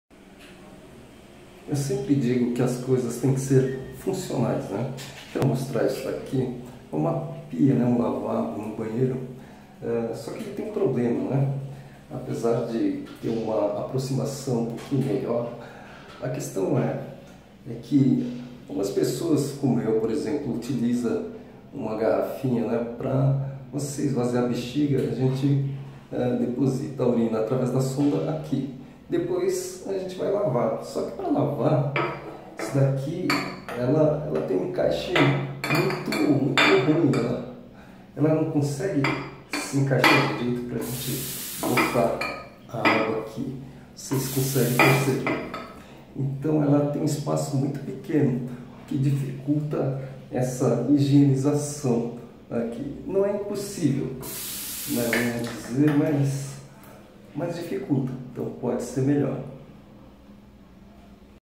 Lavatório e torneira apertados não são acessíveis
O problema é que em diversos modelos de pias, não consigo ou tenho muita dificuldade em colocar a boca da garrafinha embaixo da torneira. Não é um procedimento comum para uma pia de banheiro, seria mais para uma pia de cozinha. Mas pessoas com deficiência, muitas vezes fazem o a coleta da urina utilizando uma garrafinha, este é um hábito diferente da maioria, mas muito comum para quem utiliza banheiros adaptados.

Entenda melhor esse procedimento e a dificuldade deste tipo de lavatório